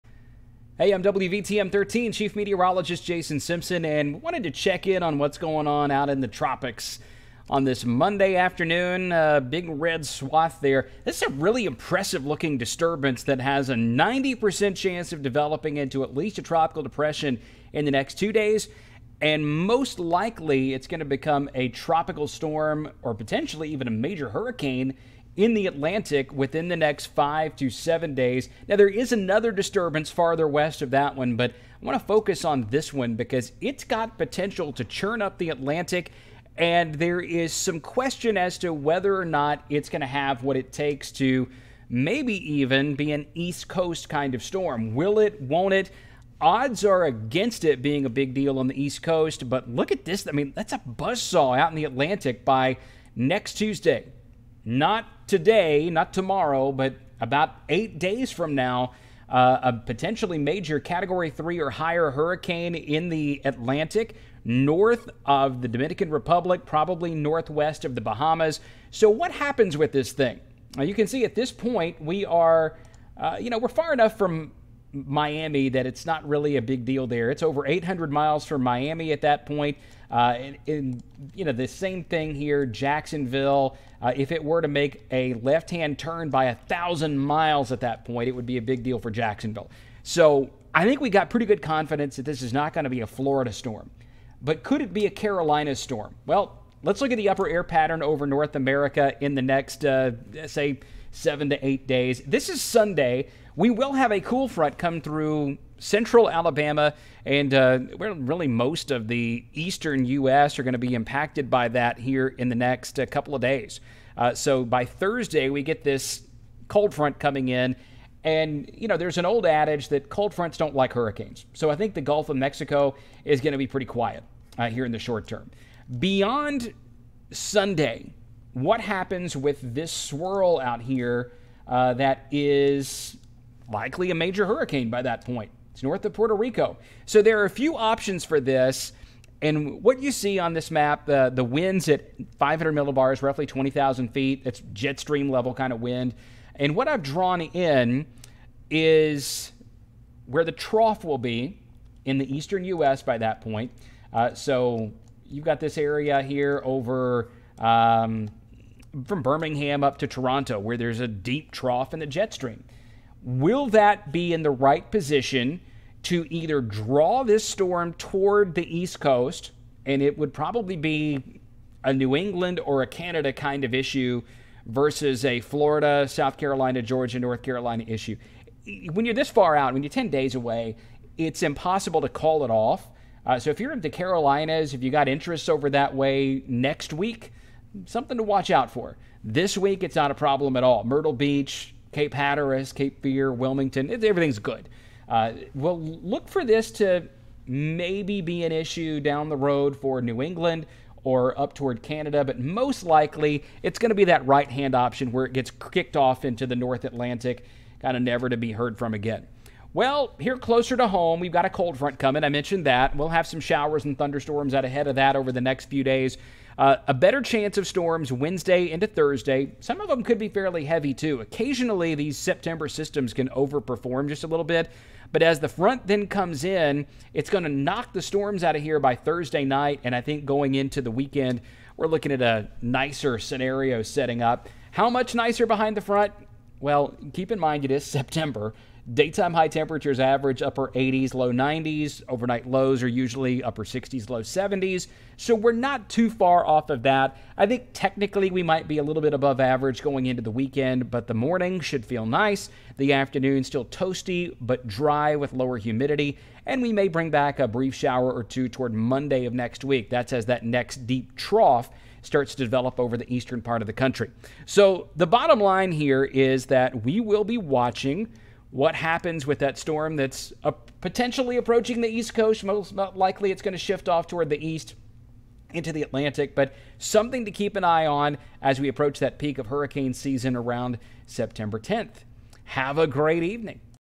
Invest 95L could grow into another powerful hurricane in the tropical Atlantic
A strong disturbance in the Atlantic on Monday, September 4th could grow into a powerful hurricane by the weekend. It is too soon to know if it will impact the East Coast or stay out to sea.